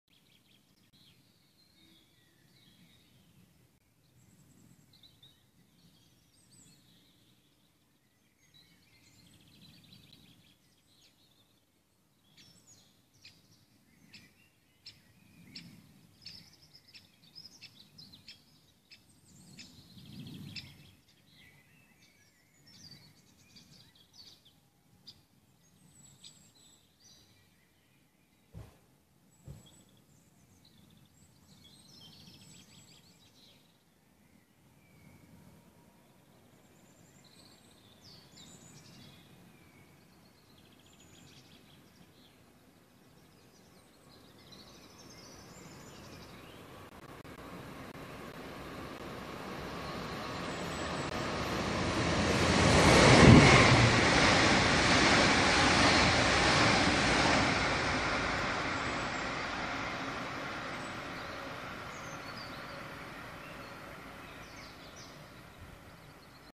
Metronom verlässt Tostedt in Richtung Bremen
Mit einem älteren Mobiltelefon aufgenommen, daher die schlechte Bild- und Tonqualität.
Reines langweiliges Zeitdokument.